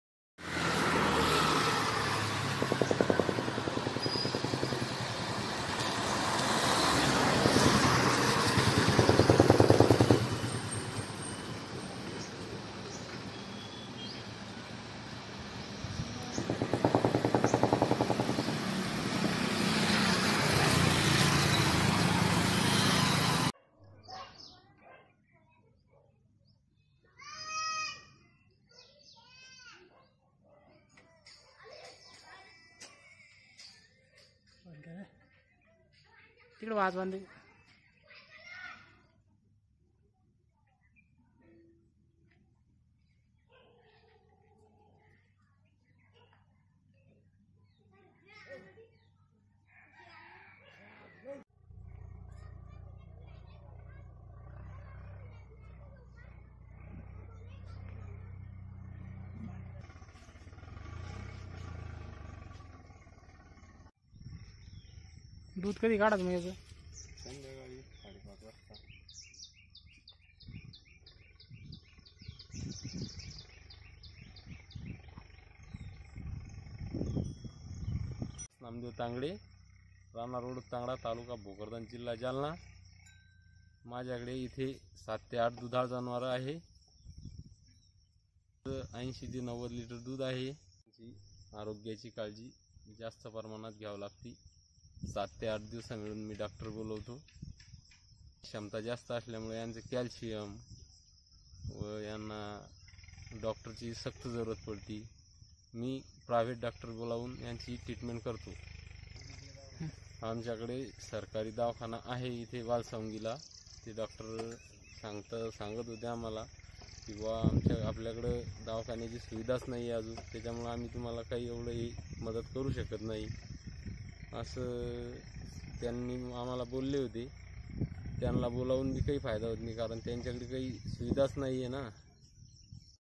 पशुसंवर्धन विभाग करतेय शेतकर्‍यांच्या जनावरांच्या आरोग्याकडे दुर्लक्ष.
जालना :- जिल्हात पशुधन बर्‍या पैकी आहे त्यामुळे या जनावरांकडे मानवी आरोग्या सारखे लक्ष द्यावे लागते ,विशेष बाब म्हणजे दुधाळ जनावरांची प्रत्येक हप्त्याला तपासणी करावी लागती ती सरकारी दवाखान्यात होणे गरजेचे आहे पण हे दवाखाने फक्त नावालाच असून सर्व तपासण्या खाजगी डाॅक्टर कडुन करुन घ्याव्या लागतात.हे कुठे तरी थांबायला पाहीजे व संबधित विभागाने चांगल्या सेवा द्याव्या अश्या चर्चा पशुपालकांत होत आहे.